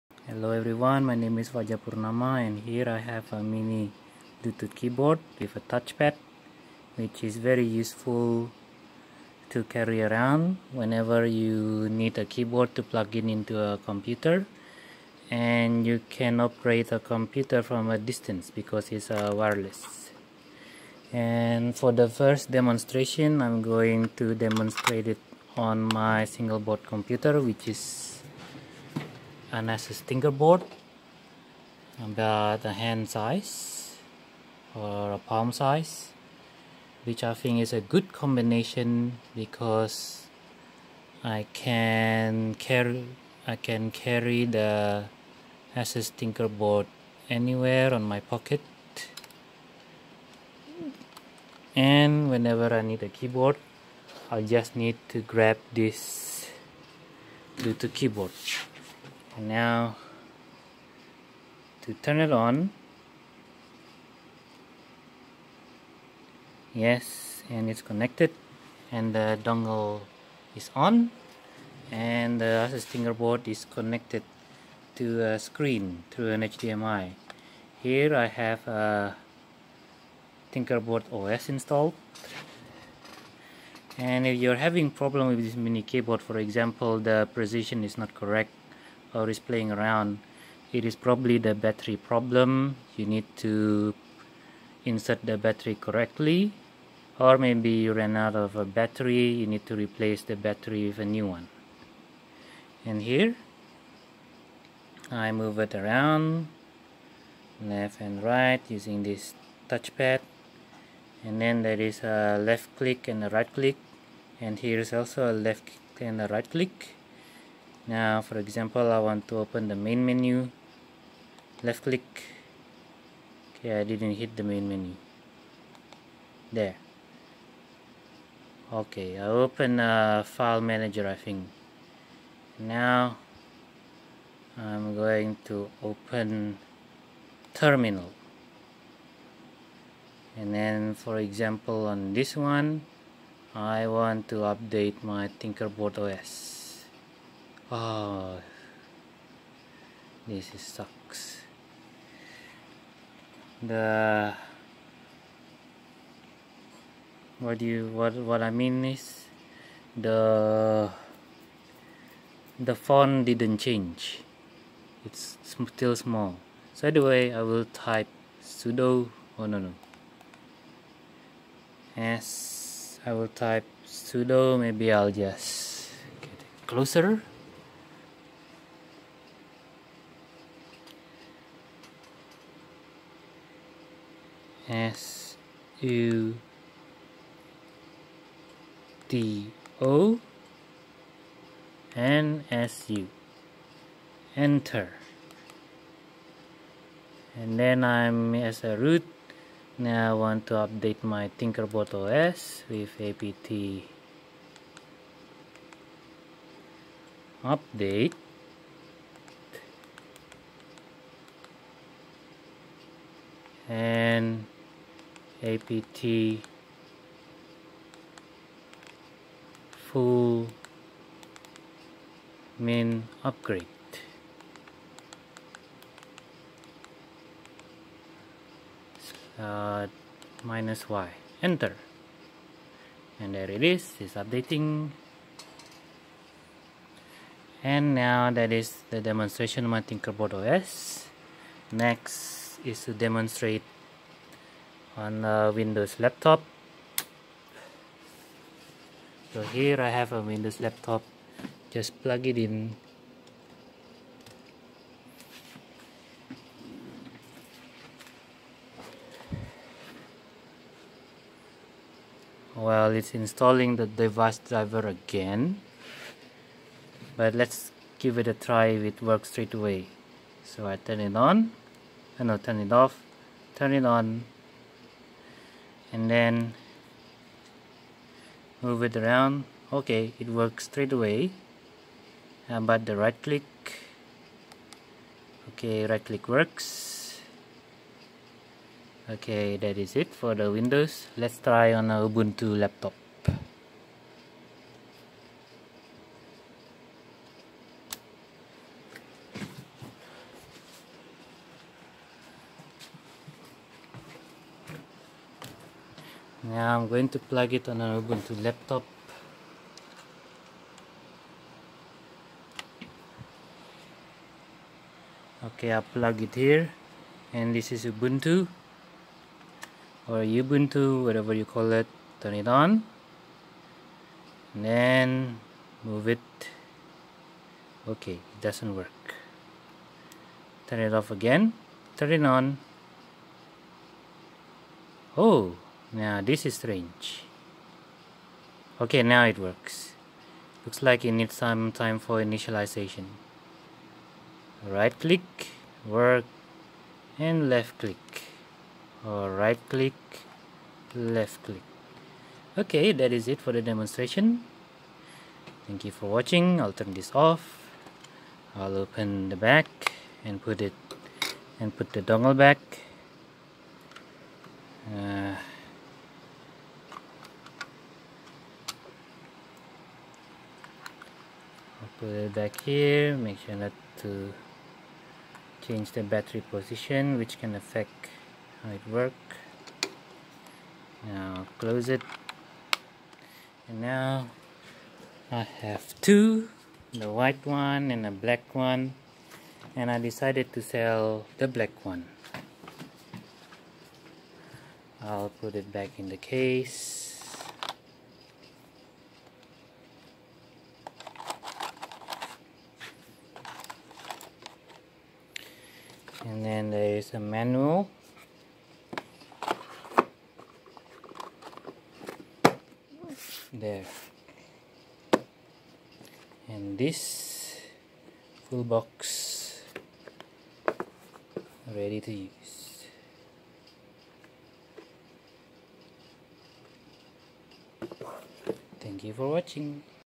Mini Wireless Keyboard with Asus Tinker Board
I demostrated using my wireless mini keyboard on my single board computer which is Asus Tinker Board. It's small enough to put inside my pocket where I usually carry it everywhere alongside my raspberry pi. It can be used on regular computers as well.